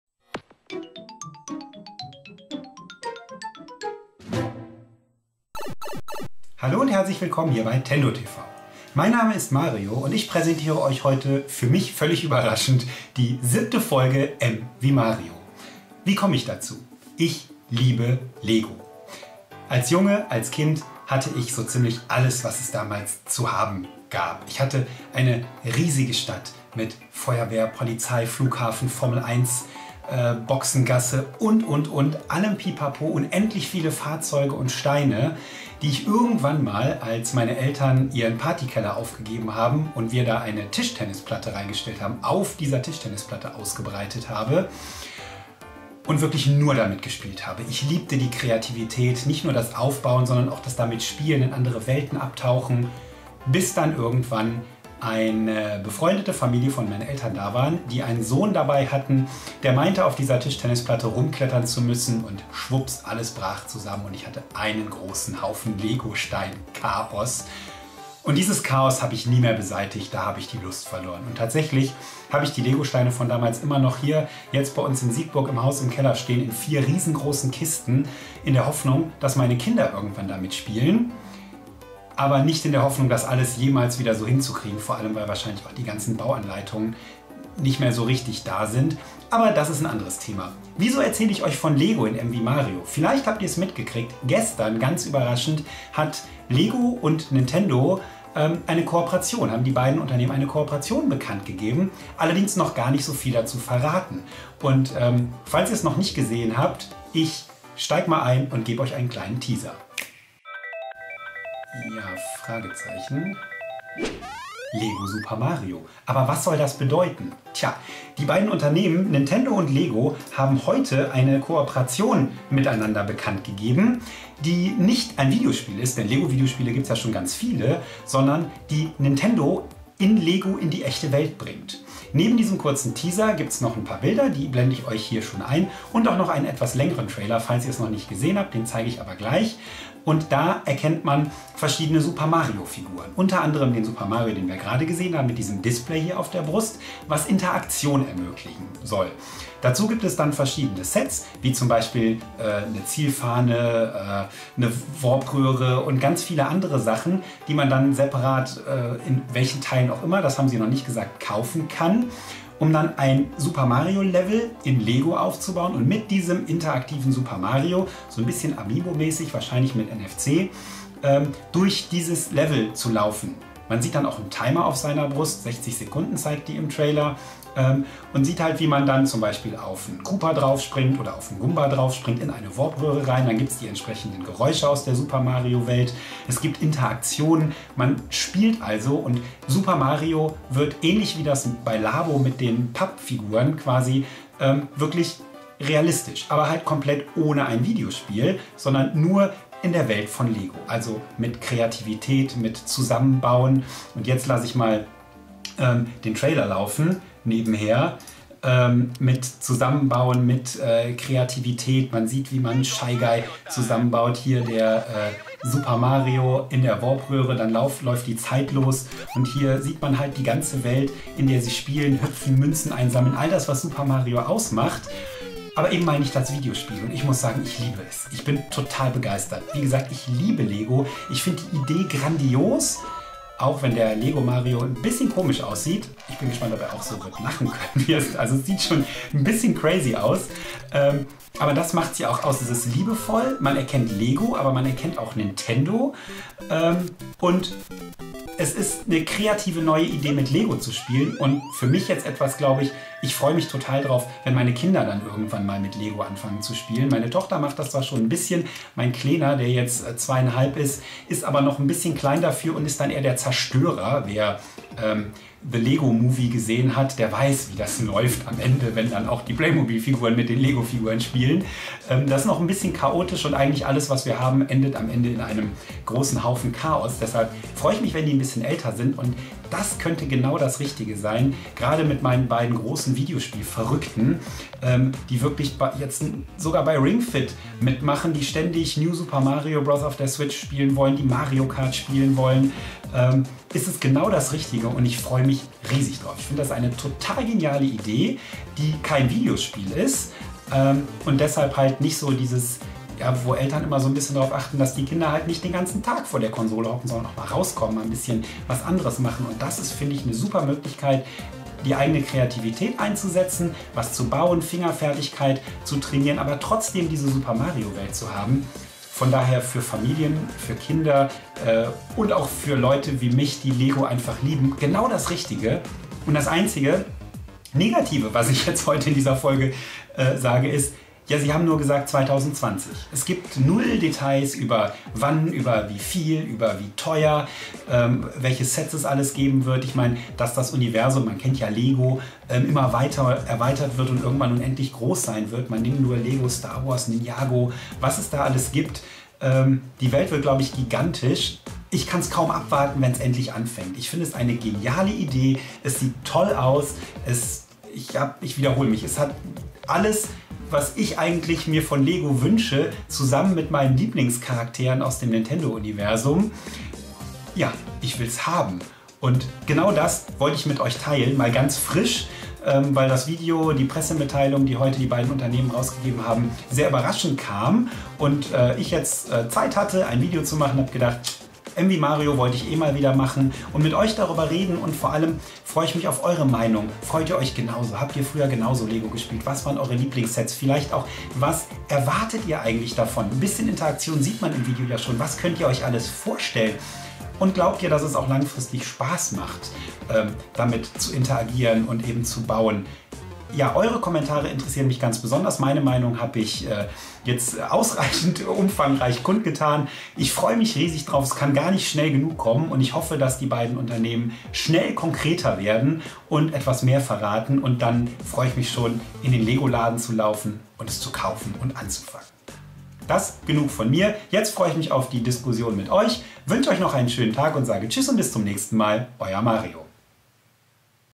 Hammer: LEGO Super Mario | M wie Mario
Gestern haben Nintendo und LEGO eine Kooperation angekündigt. Das Ergebnis: Super Mario als LEGO-Welt. Ein mir bisher unbekannter Traum wird damit wahr.